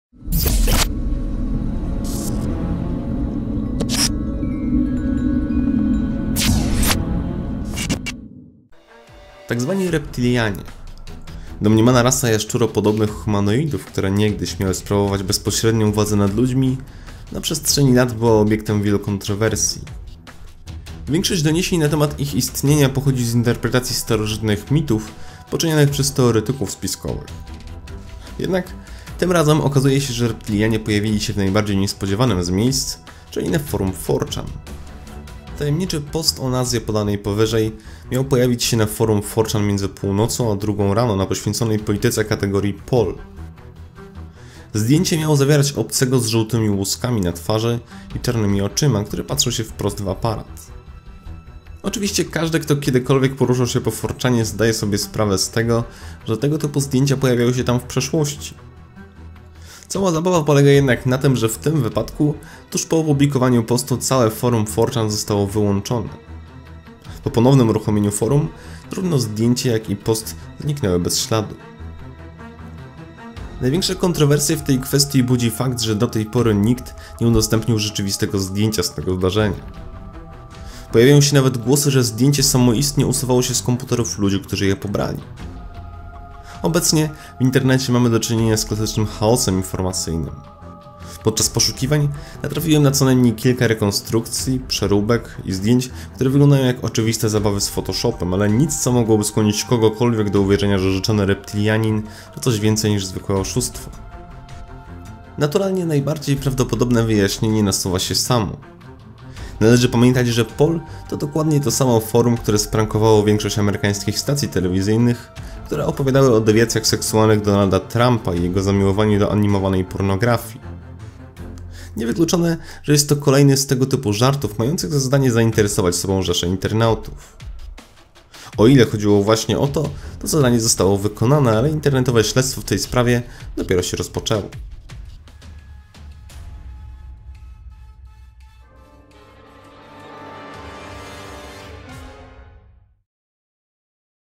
Domniemane zdjęcie reptilianina pojawiło się na forum 4chan
paranormalne Reptilianie, domniemana rasa jaszczuropodobnych humanoidów, które niegdyś miały sprawować bezpośrednią władzę nad ludźmi, na przestrzeni lat, była obiektem wielu kontrowersji. Większość doniesień na temat ich istnienia pochodzi z interpretacji starożytnych mitów, poczynianych przez teoretyków spiskowych. Jednak tym razem okazuje się że Reptilianie pojawili się w najbardziej niespodziewanym z miejsc czyli na forum 4 chan.